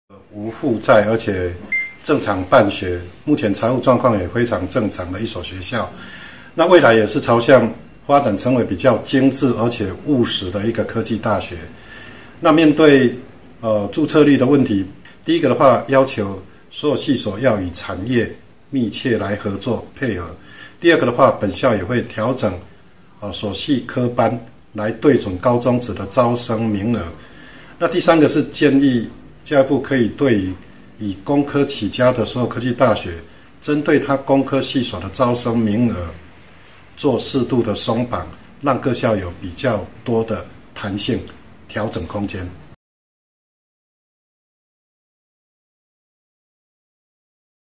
搶救新生註冊率 台南南榮科大整併科系加強產學合作
台南鹽水南榮科技大學106學年度新生註冊率32.17％，全國倒數第二。校長王俊惠今天說，將把目前14個學系在107學年度整拼為11個學系，並加強產學合作，朝小而美的科大發展，提升註冊率。